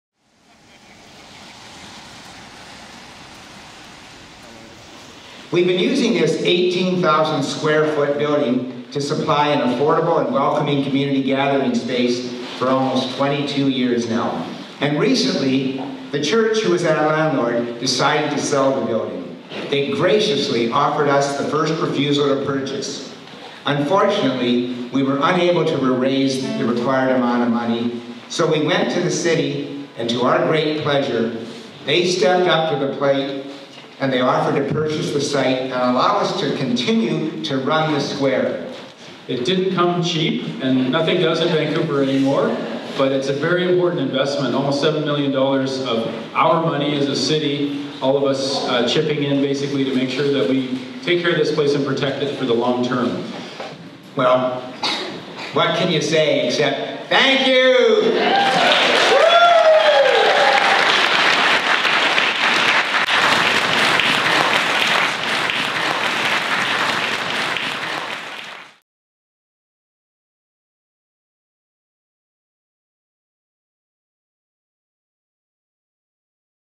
St James Hall saved!
On February 13, 2016, there was a gathering in St James Square in which the city announced that it would buy building and lease it buy to  the non-profit that was running it as a community centre.